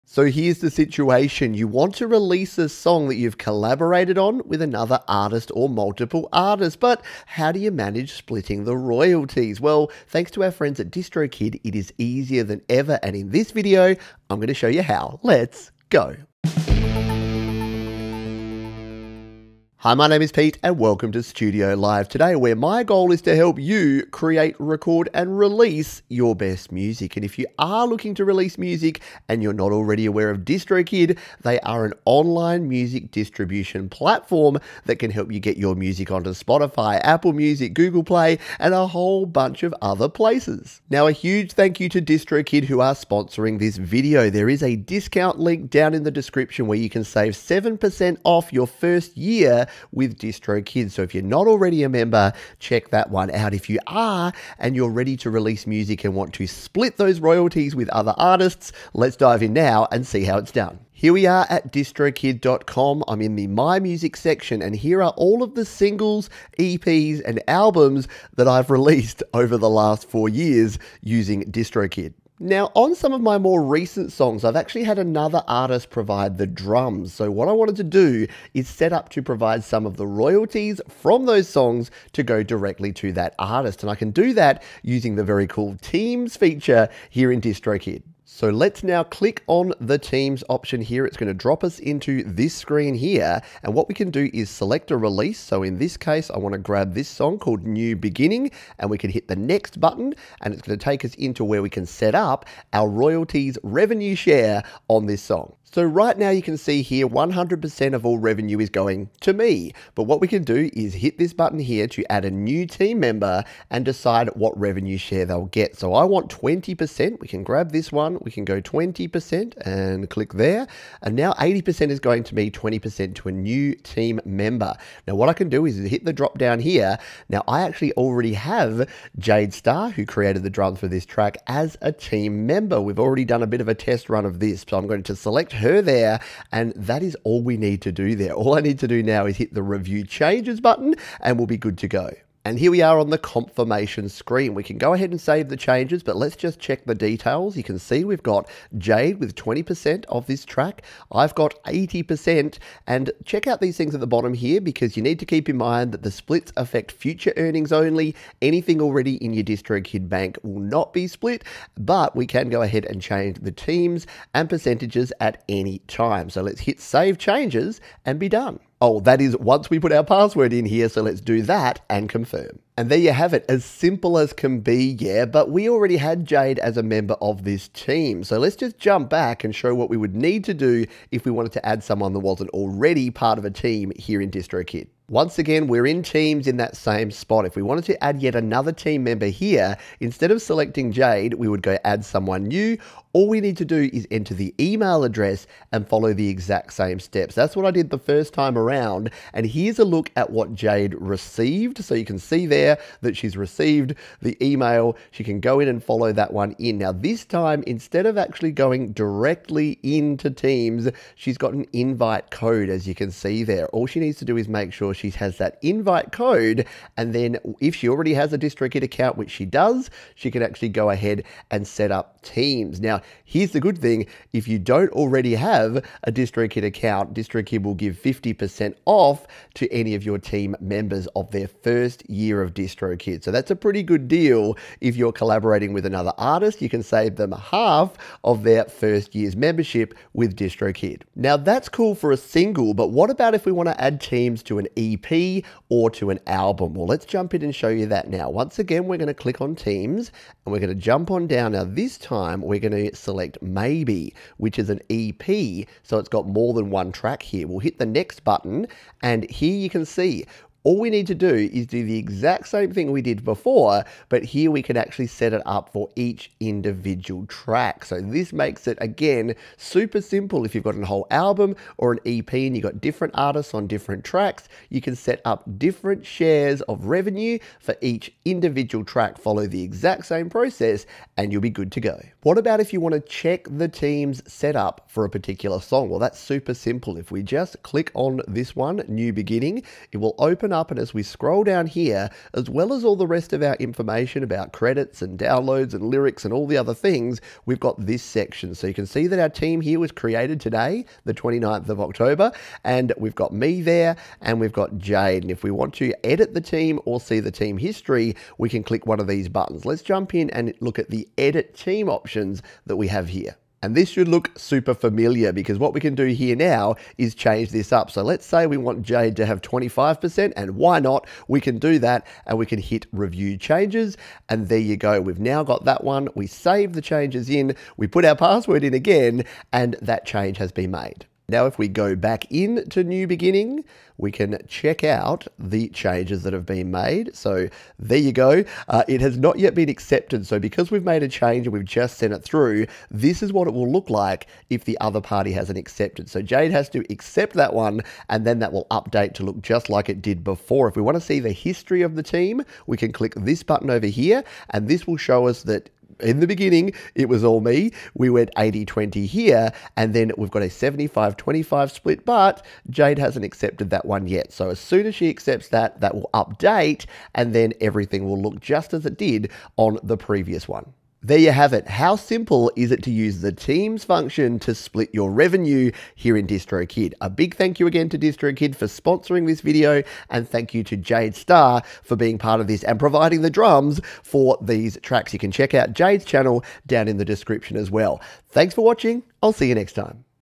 How to SHARE ROYALTIES with DistroKid | Teams tutorial
How to use DistroKid's TEAMS feature to share revenue and royalties with featured artists and collaborators when releasing music online.

⏰ Time Stamps:
00:00 What is DistroKid?
01:01 How to split revenue
03:05 How to add a new team member
04:01 Split revenue for an album/EP
04:42 Edit team
05:50 View team history